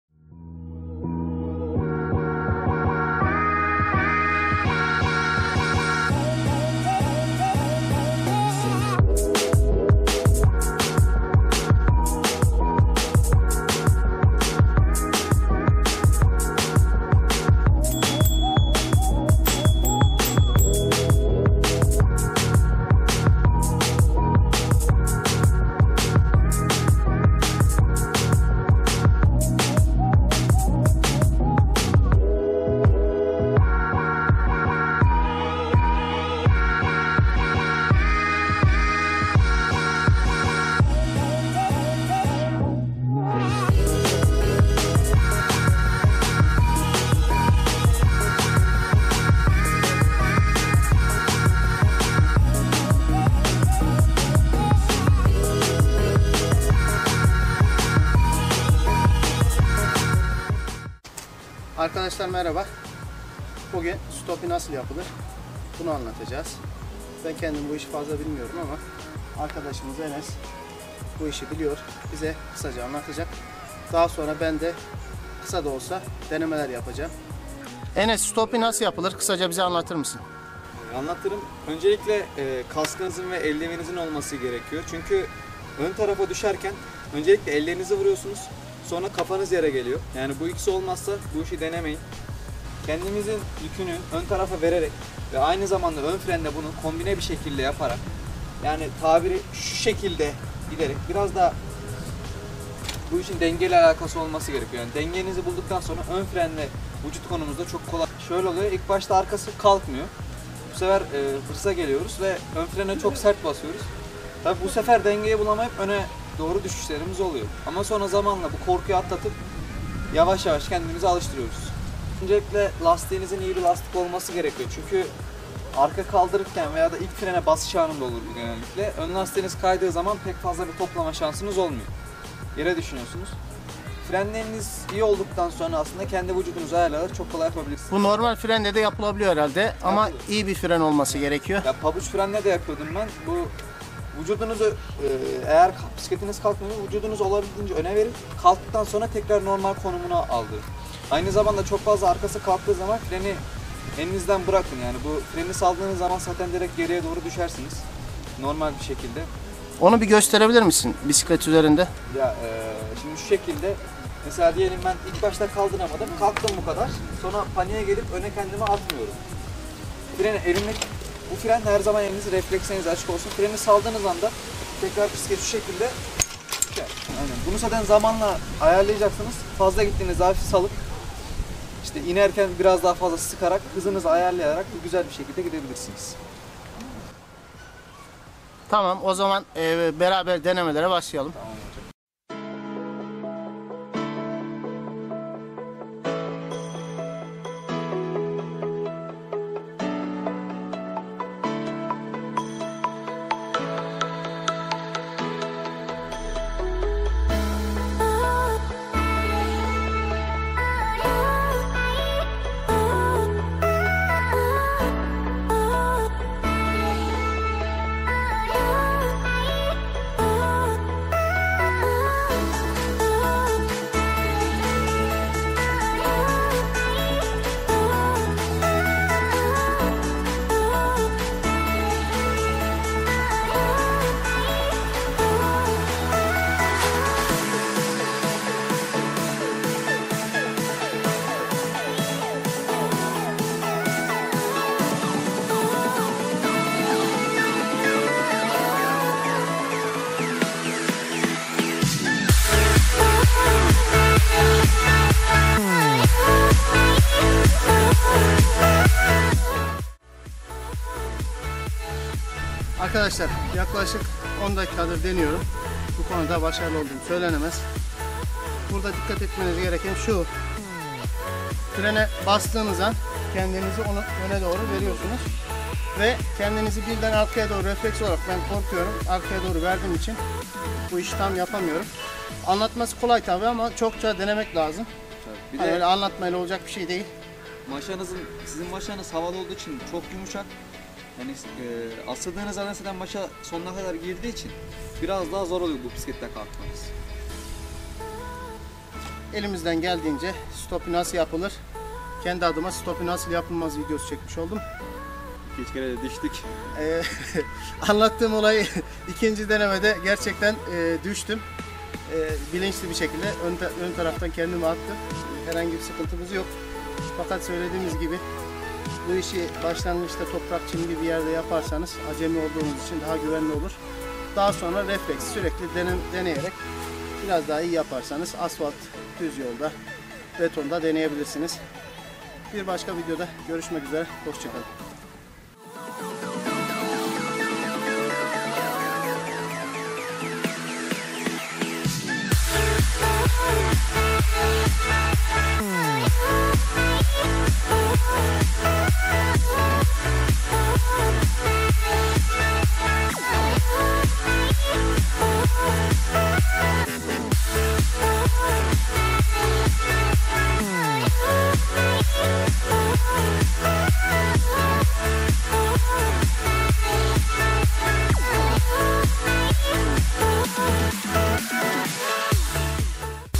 Stoppie Nasıl Yapılır? Nasıl Yapılmaz? Bisiklette Arka Kaldırma/ Denedik Uçtuk:)
Merhaba, bu videomuzda bisiklette stoppie(arka kaldırma) nasıl yapılır onu anlattık. İyi Seyirler